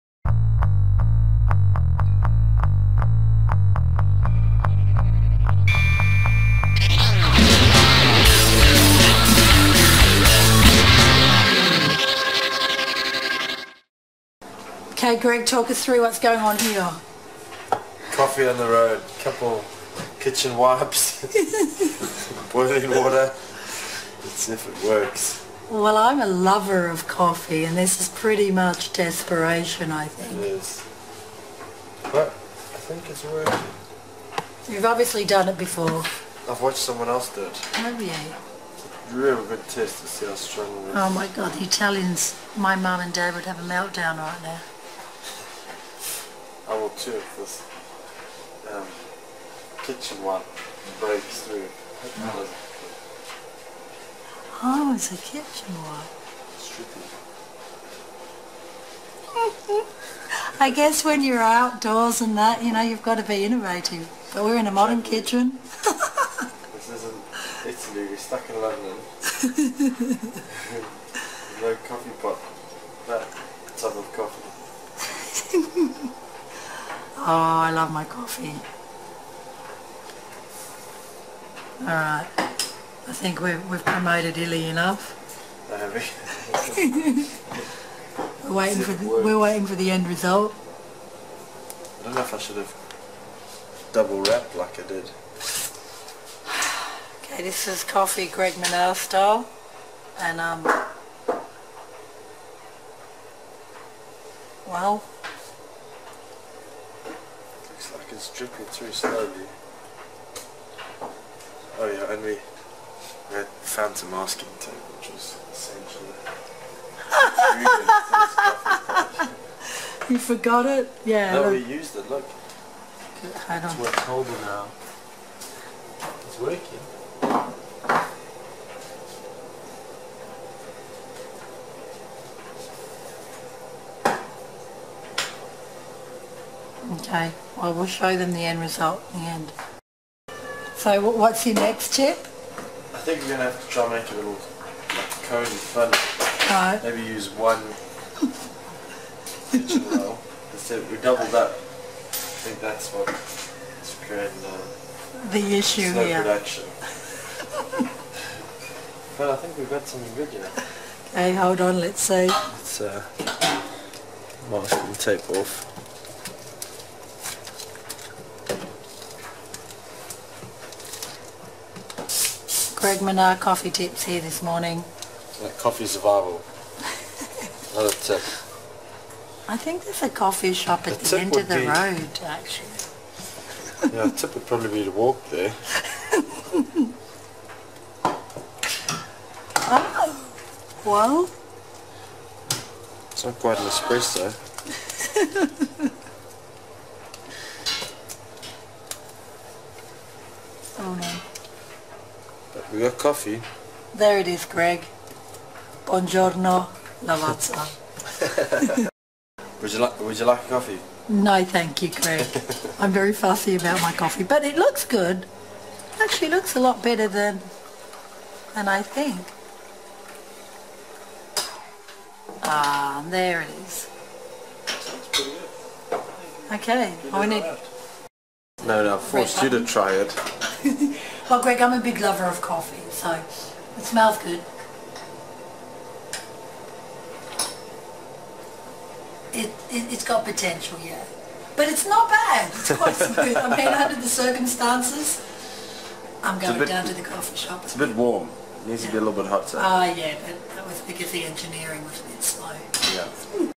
Greg Minnaar - Mountain Biker Shares His Coffee Tips
Greg demonstrates when all is desperate there are ways of making coffee .